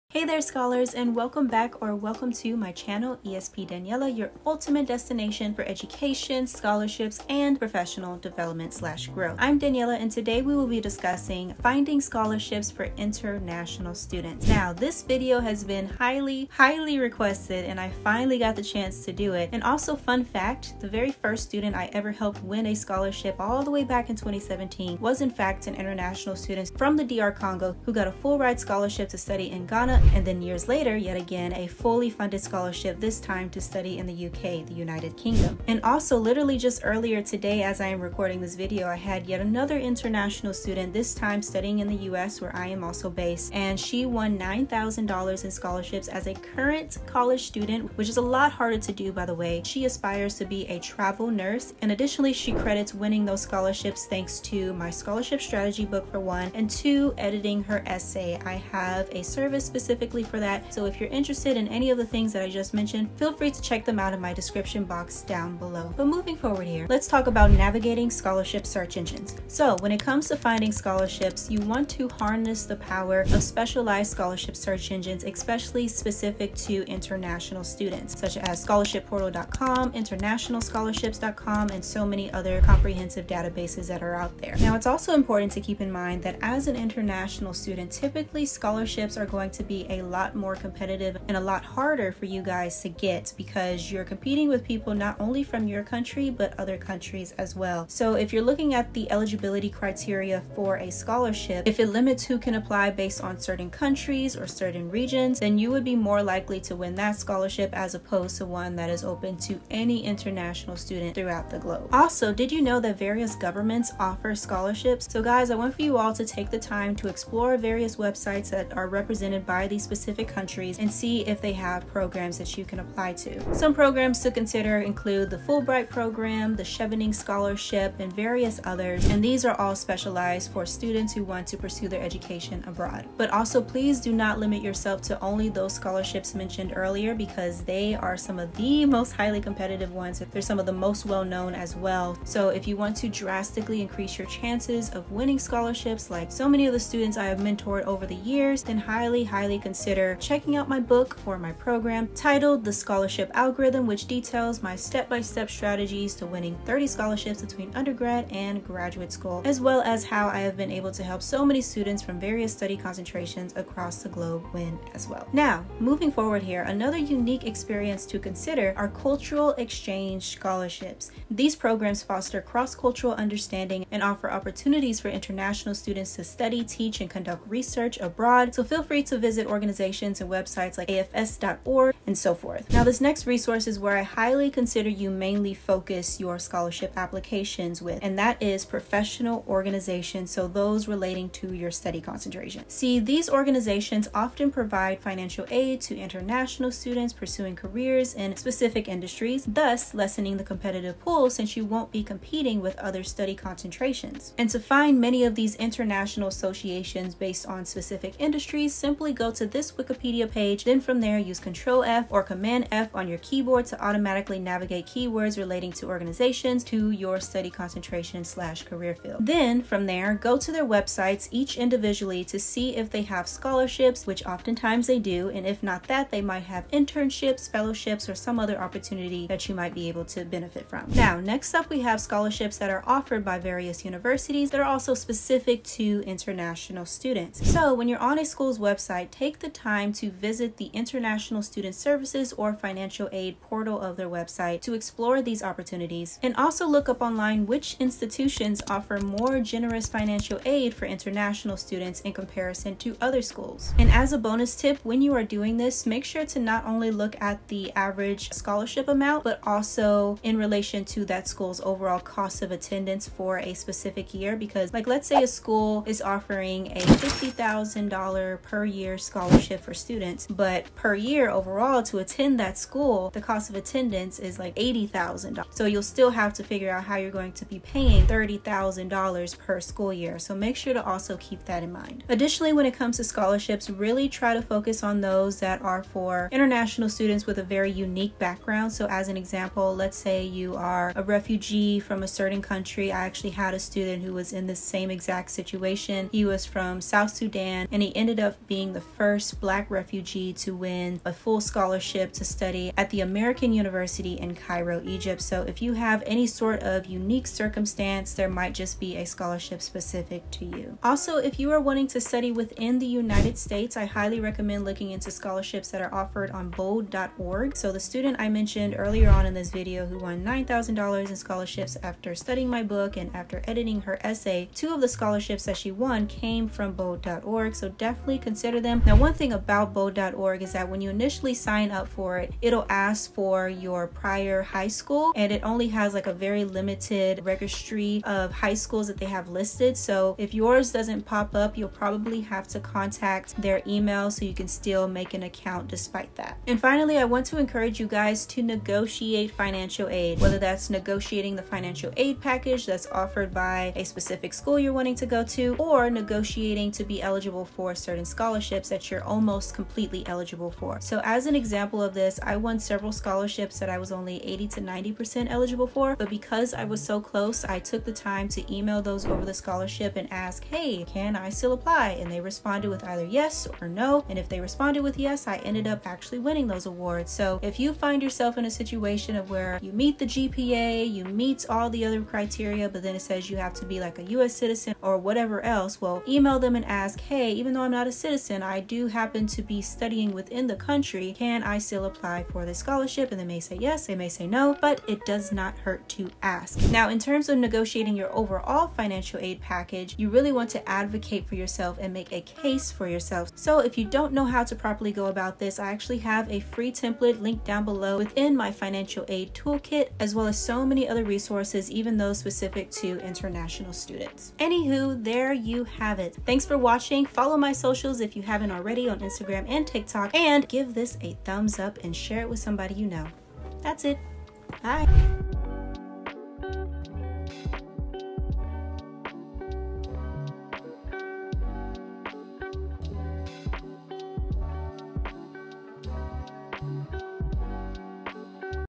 International Student Scholarship Expert Advice | Fully Funded & Partial Awards
Having helped international students studying in the U.S. and other countries as well win anywhere from partial to full-ride scholarships since 2017, I wanted to share my tips for YOU to hopefully see similar results.

⏰ TIME STAMPS:
00:00 -